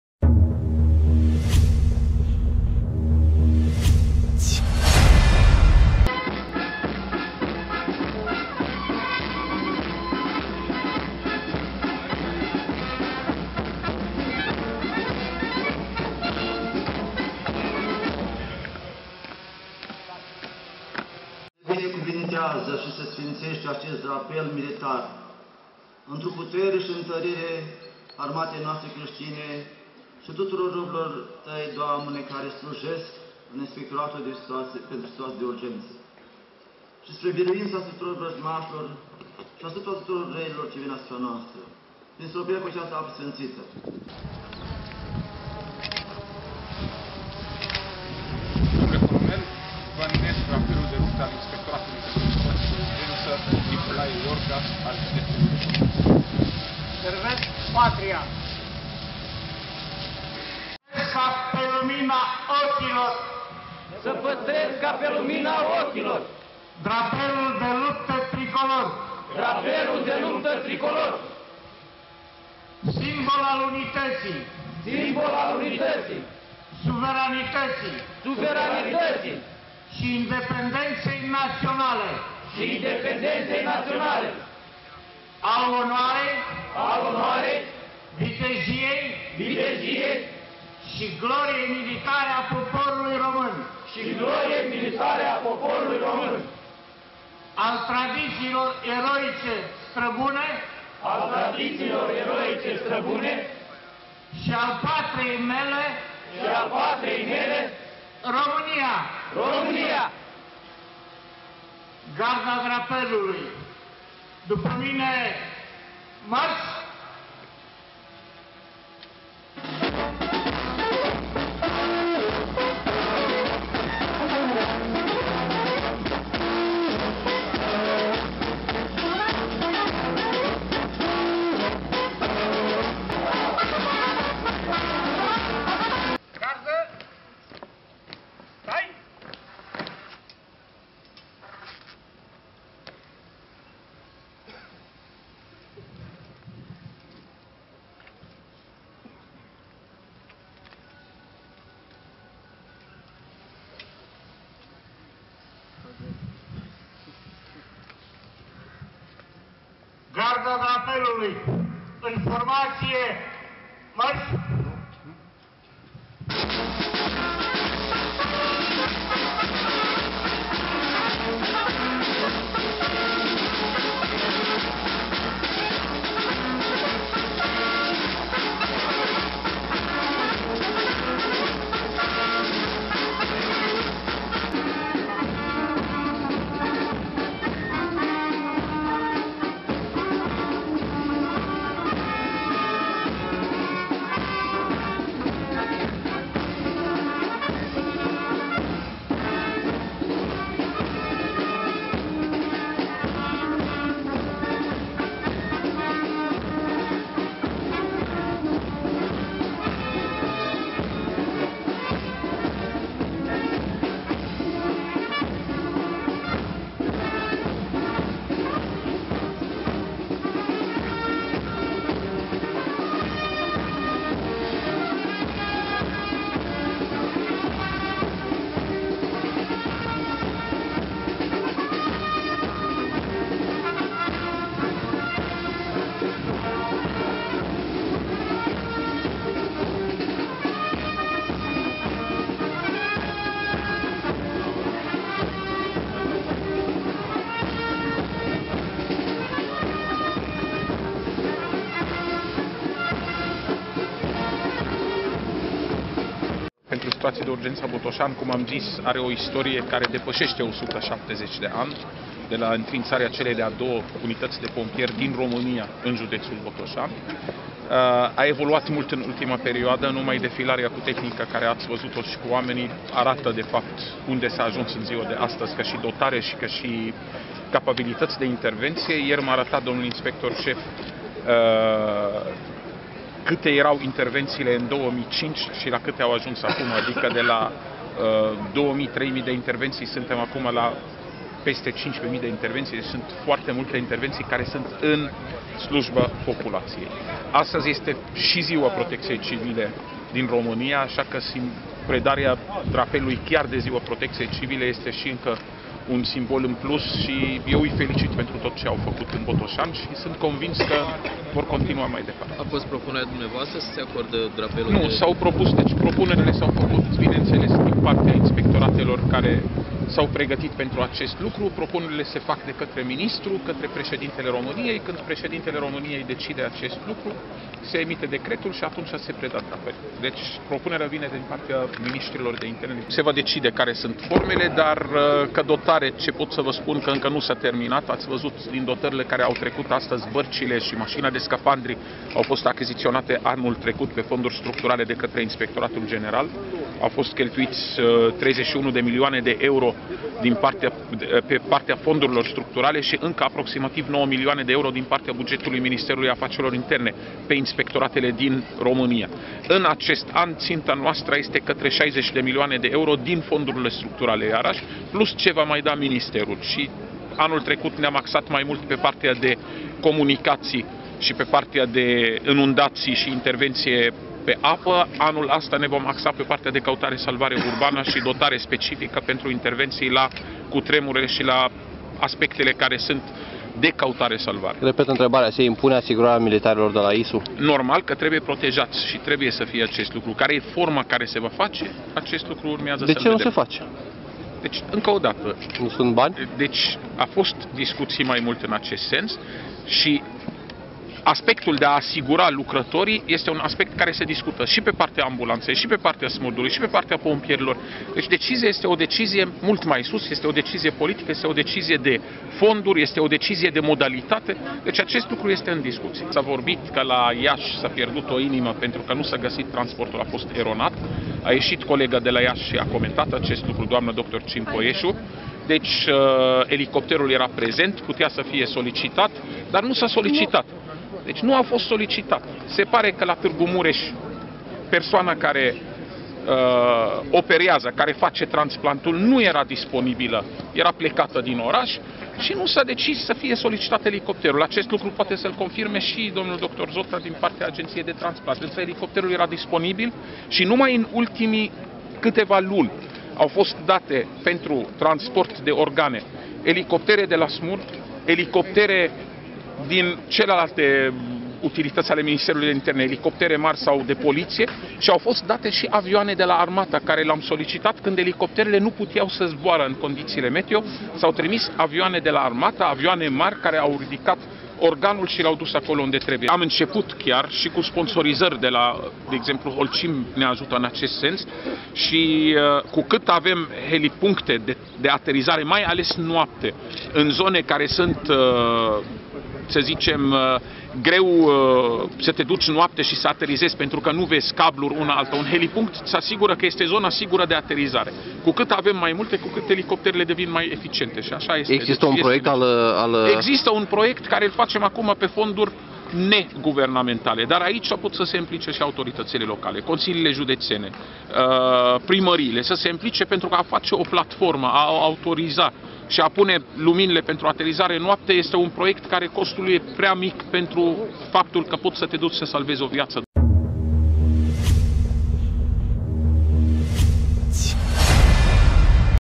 ISU Botoșani a primit drapelul de luptă
Inspectoratul pentru Situații de Urgență (ISU) al județului Botoșani a primit, duminică, drapelul de luptă, în cadrul unei ceremonii organizate cu prilejul Zilei Protecției Civile